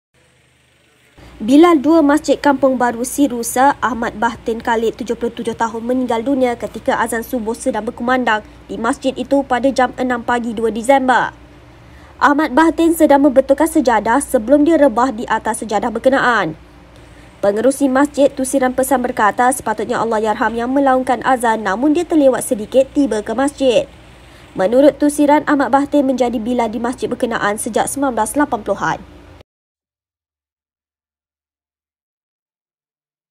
[3.12.2020] Bilal meninggal dunia ketika azan Subuh berkumandang
Bilal 2 Masjid Kampung Baru Si Rusa dekat sini, Ahmad Bahtin Khalid, 77, meninggal dunia ketika azan Subuh sedang berkumandang di masjid ini, jam 6 pagi 2  Disember. Arwah sedang membetulkan sejadah sebelum dia rebah di atas sejadah berkenaan.

Sumber: Harian Metro